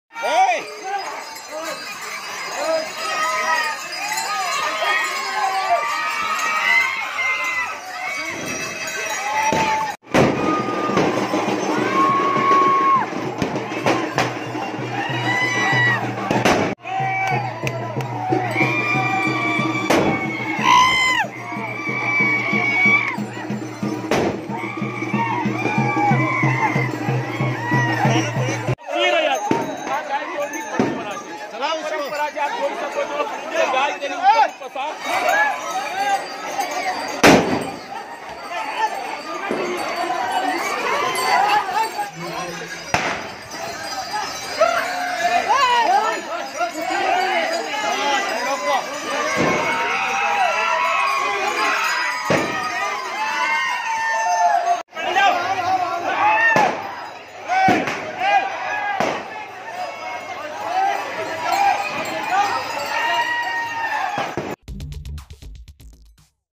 Villagers allow cows and bulls to run over them in Gujarat's Gaai Gohri festival
In a traditional ritual, people in Gujarat's Dahod district allow cows and bulls to run over them every year on second day after Diwali when they celebrate Gaai Gohri festival.